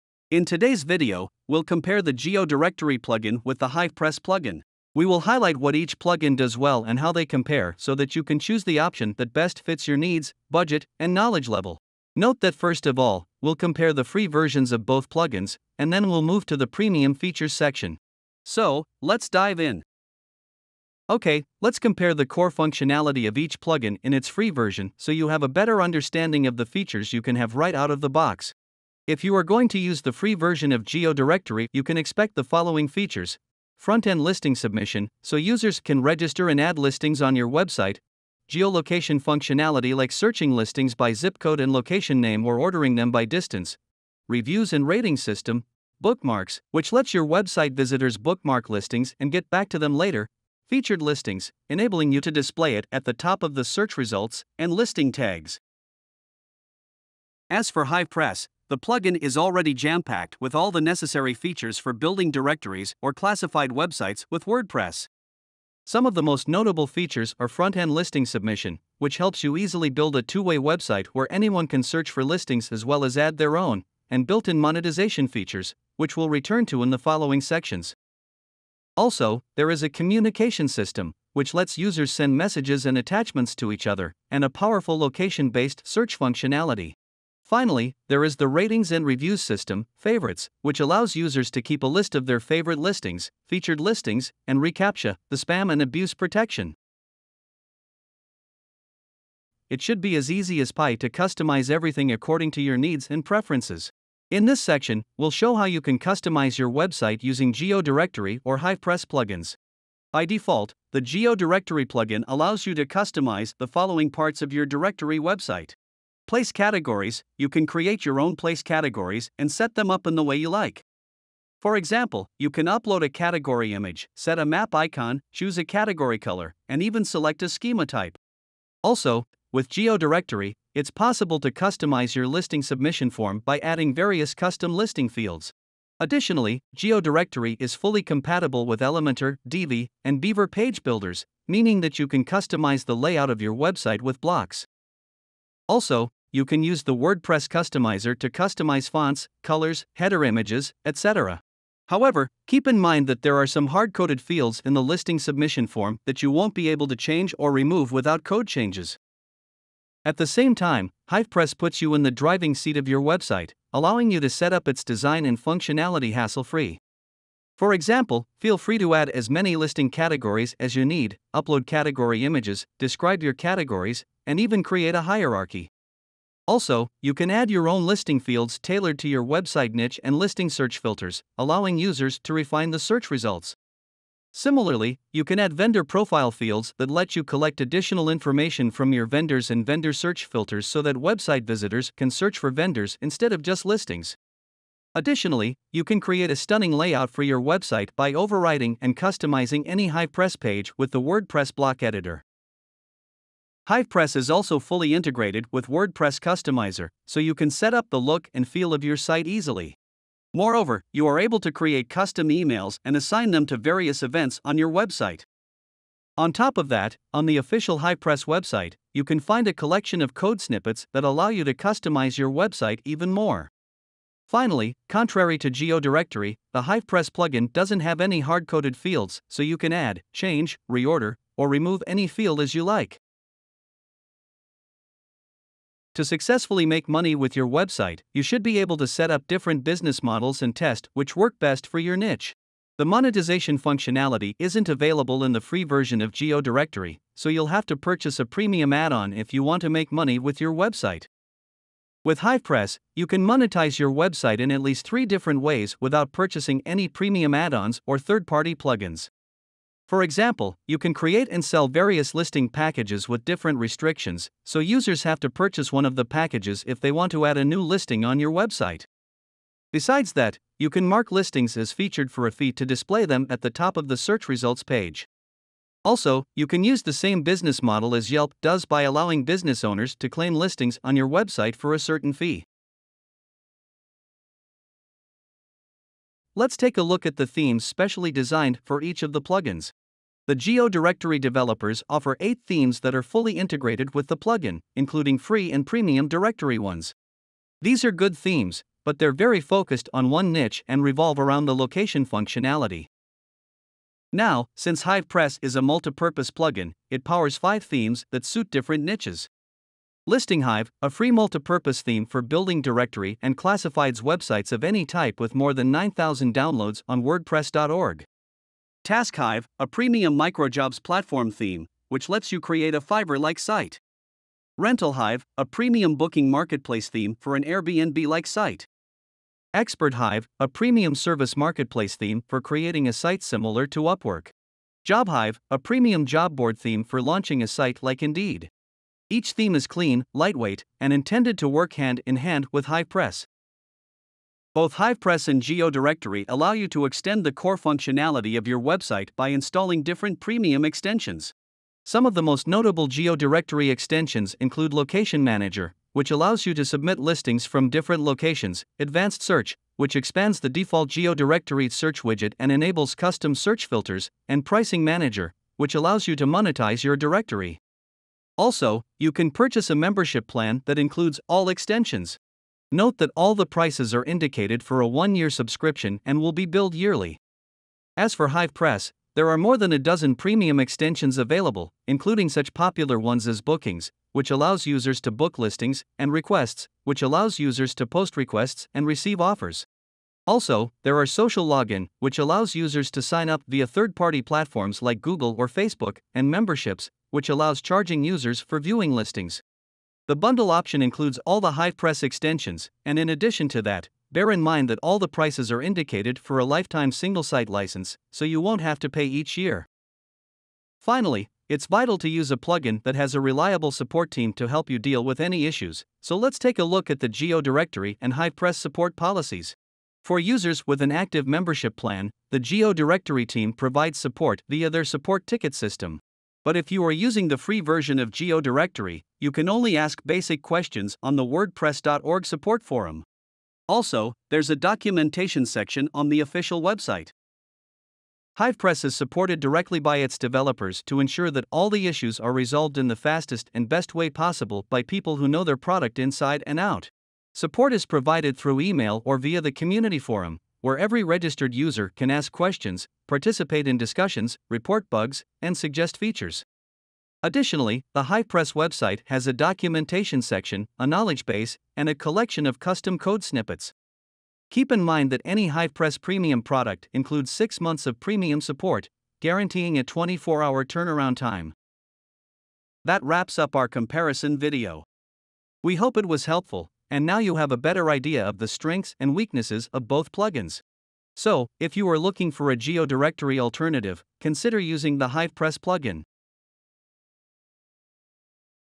GeoDirectory vs HivePress Business Directory WordPress Plugin [Comparison]
In this video, we’ll compare side by side the main features of GeoDirectory and HivePress plugins, their customization & monetization options, the support policies and so on.